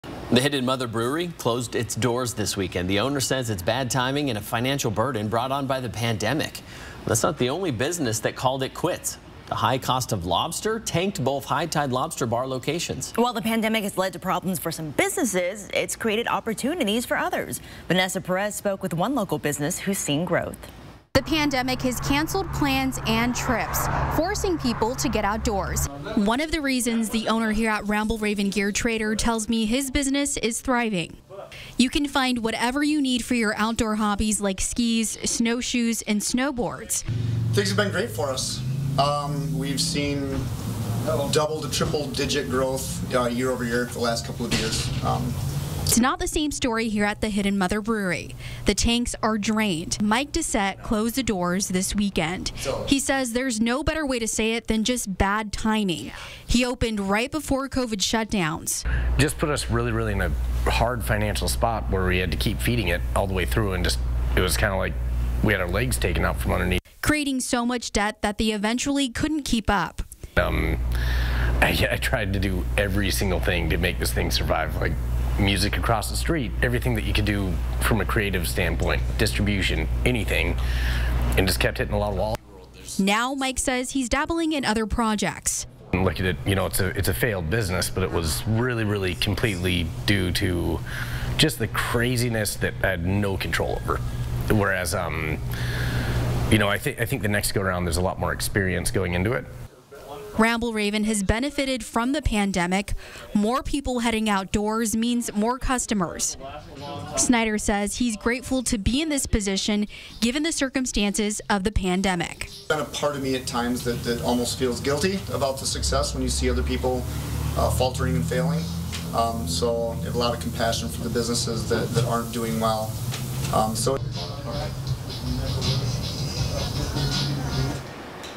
COVID-19 affects local businesses
The financial burden put on some businesses by the pandemic has forced them to close.

The high cost of lobster tanked both High Tide Lobster Bar locations, owner Chad White announced on Sunday. The Hidden Mother Brewery closed its doors as well. Owner Mike Detar says there’s no better way to put it other than due to bad timing.

The location blocks away from the Spokane Arena opened right before COVID shutdowns.

“Just put us in a hard spot where we had to keep feeding it all the way through, and just it was kind of like we had our legs taken out from underneath us,” owner and founder of The Hidden Mother Brewery Mike Detar said.

Detar says they weren’t able to get enough people in seats because of the shutdowns. He says they also couldn’t meet their minimums and it buried the brewery in debt.

“I tried to do every single thing I could to make this thing survive like music across the street, everything you could do from a creative standpoint, distribution-anything. Just kept hitting a lot of walls and at some point, you’ve got to stop,” Detar said.

Detar says he’s now dabbling in other projects.

“I don’t even look at it. It’s a failed business but it’s really, really completely due to just the craziness of stuff I had no control over whereas I think the next go around there’s a lot more experience going into it,” he said.

He hopes the person who takes on the location can reap the benefits they built.

While the pandemic has created numerous problems for businesses, some have been fortunate to thrive off the pandemic. Such as Rambleraven Gear Trader who says the pandemic has canceled long-term plans and trips forcing people to want to get outdoors.

Rambleraven operated solely as a consignment store for its first three years, and then in 2019, the owner expanded to retail and service.

“Things have been great for us we’ve seen double to triple digits growth year over year over the last couple of years,” Mike Schneider owner of Rambleraven Gear Trader said.